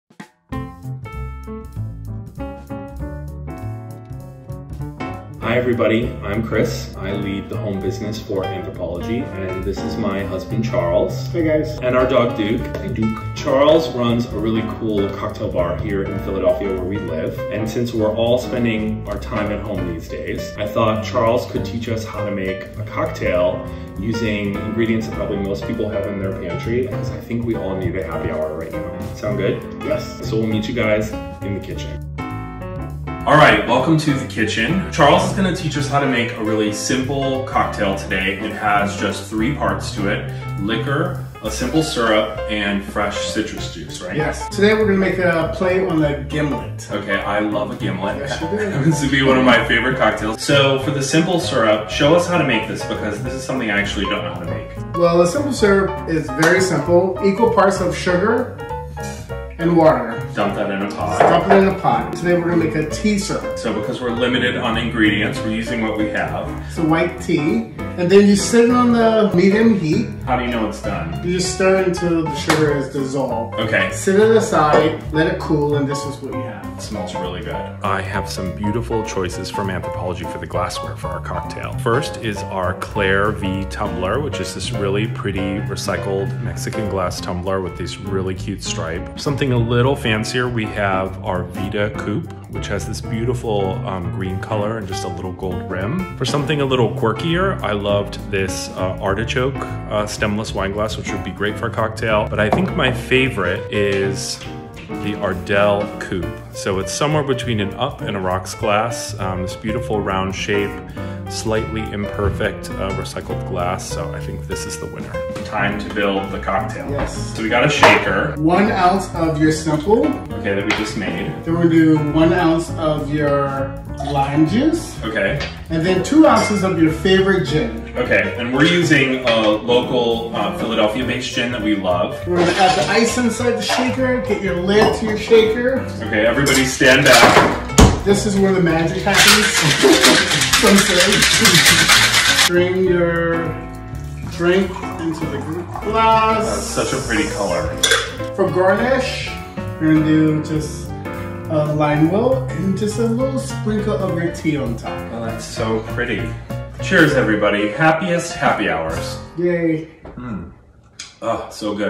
Happy Hour at Home
On the fifth episode of our Afternoons With Anthro video series, Sr. Divisional Merchandise Manager for Home Chris Sotz and his husband are serving up their super-simple cocktail recipe.